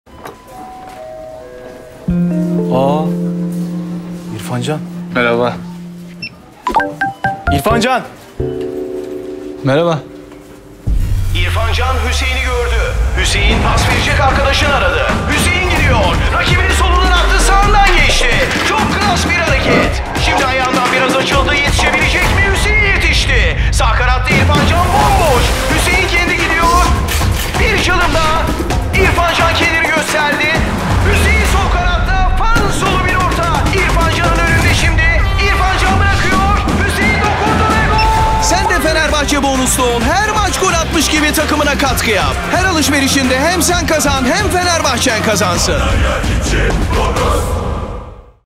Fenerbahçe Bonus’la İrfan Can’a taraftardan sürpriz katkı!
İrfancan hayatında böyle destek görmedi, markette İrfancan’la karşılaşan taraftar heyecanından markette bir futbol şölenine imza atıyor. İrfancan’a sadece dokunmak kalıyor!
Şimdi sen de Fenerbahçe Bonus’lu ol, hem sen kazan hem Fenerbahçe kazansın.
Üstelik ilk defa Bonuslu olanlara 1.000 TL bonus!